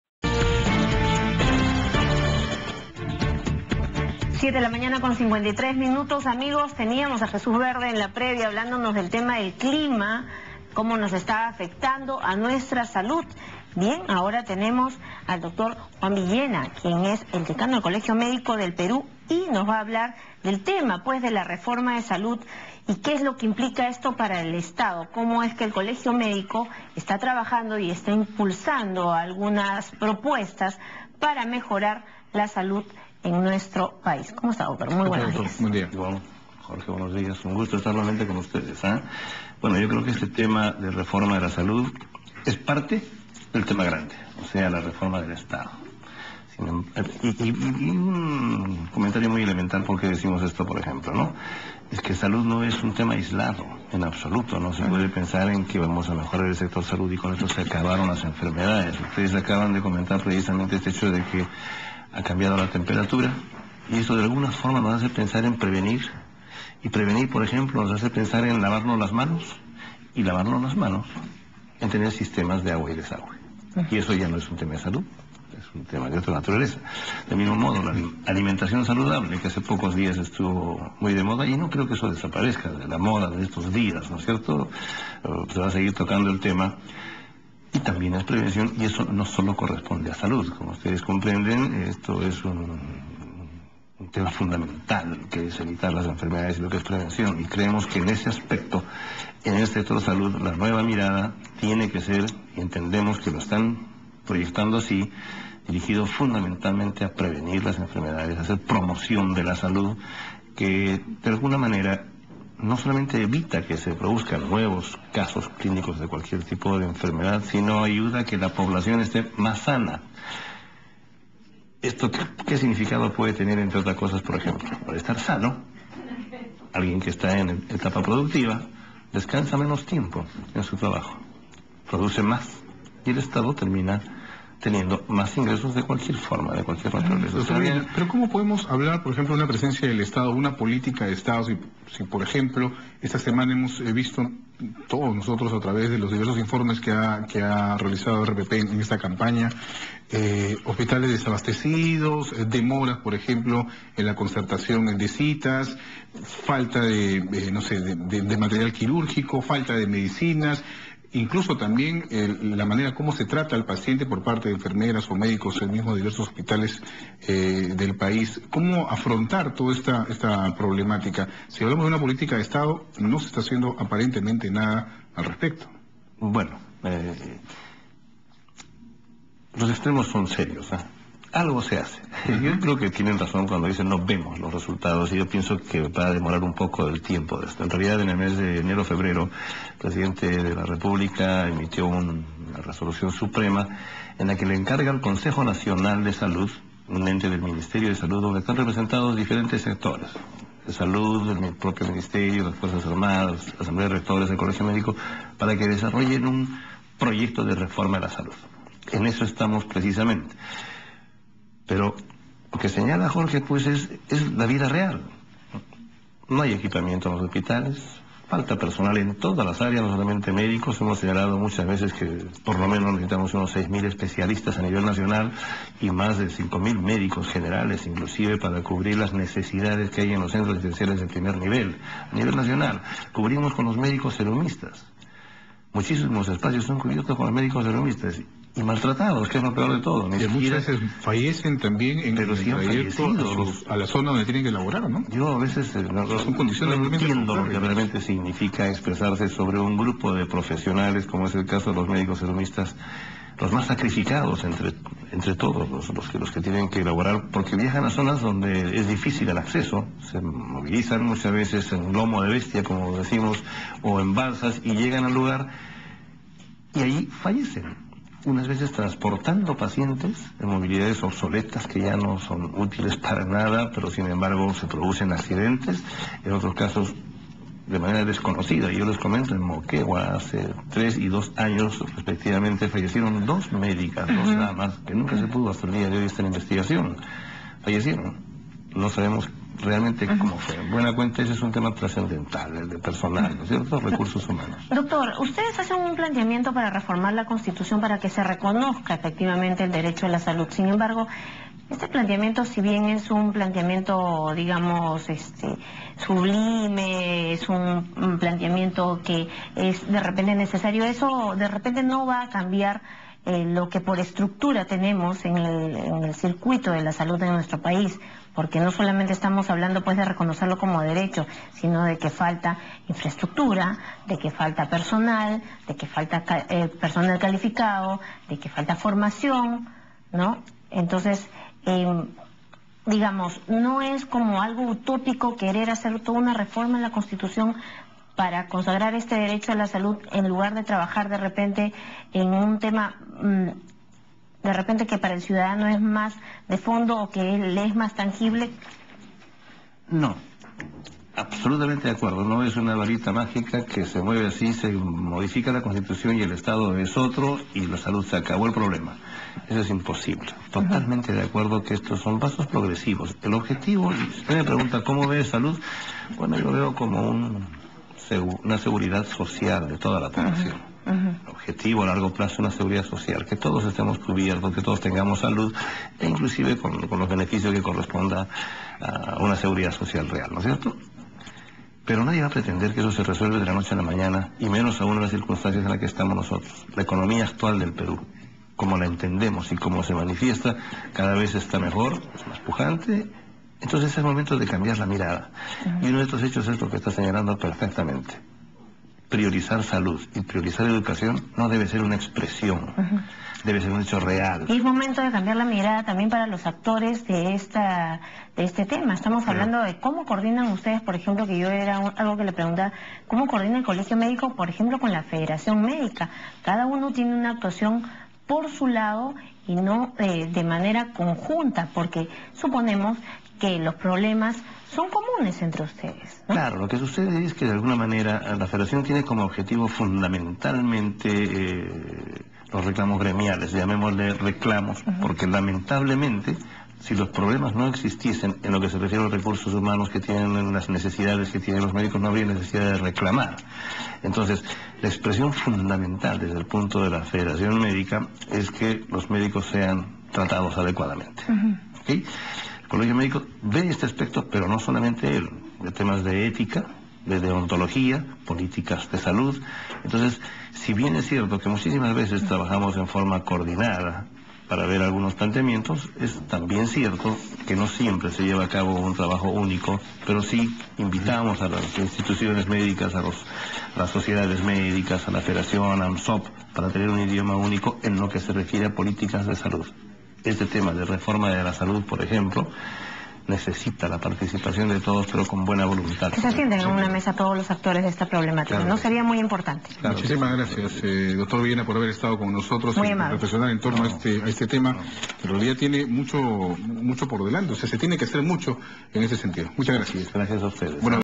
Entrevista. El Décano del Colegio Médico del Perú, Juan Villena RPP
Entrevista. El Décano del Colegio Médico del Perú, Juan Villena, indicó que la reforma del sector salud, forma parte del gran proyecto de la reforma del Estado. Asimismo, manifestó que la alimentación saludable y las actividades de prevención de enfermedades, no solo corresponden a una tarea específica del sector salud, sino que engloba una serie de esfuerzos para que se implementen estos planes. Por otra parte, acotó que tomará mucho tiempo en poder lograr un cambio dentro de este sector, pues aún se suscitan casos en hospitales en donde faltan equipos y personal, siendo la demanda nacional de cerca de 6 mil especialistas. En otras declaraciones, destacó la labor de los médicos serumistas, que muchas llegan hasta los lugares más alejados del país, pero que a pesar de ello, son maltratados por las autoridades médicas.